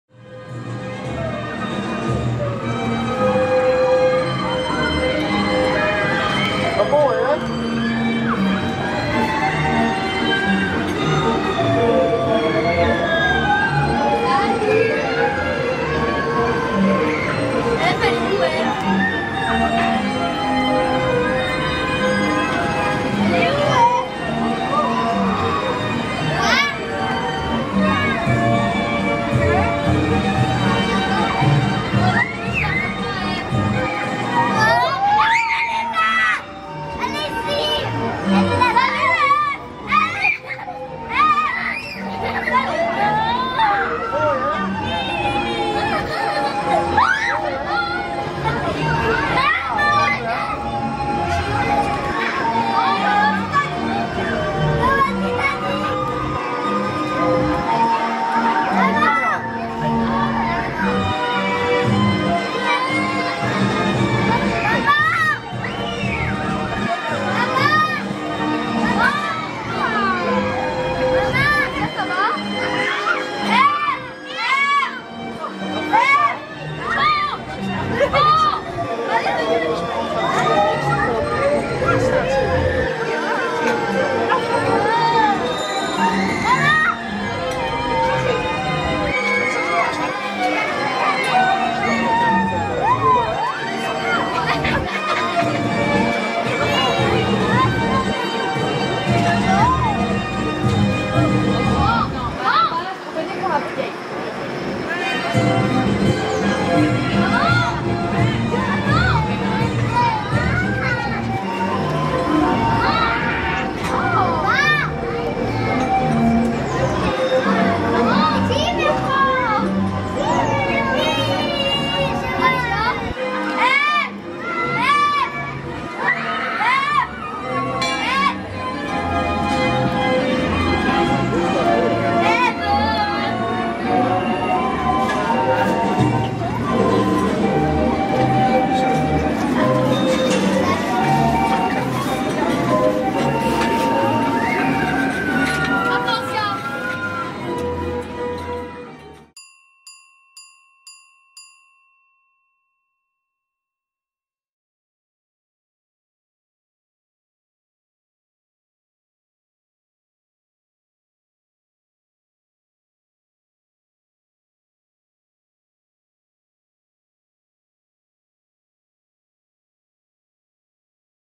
Een ritje op de Kleine Zweef! - 06-05-2023 - Efteling
Eén van de zweefmolens van de Efteling, de Kleine Zweef, is vernieuwd! De decoraties zijn opnieuw geschilderd, en er is een viertal apen toegevoegd. Ook technisch is er veel verbeterd.

(Groep Optochten in en rond Nederland)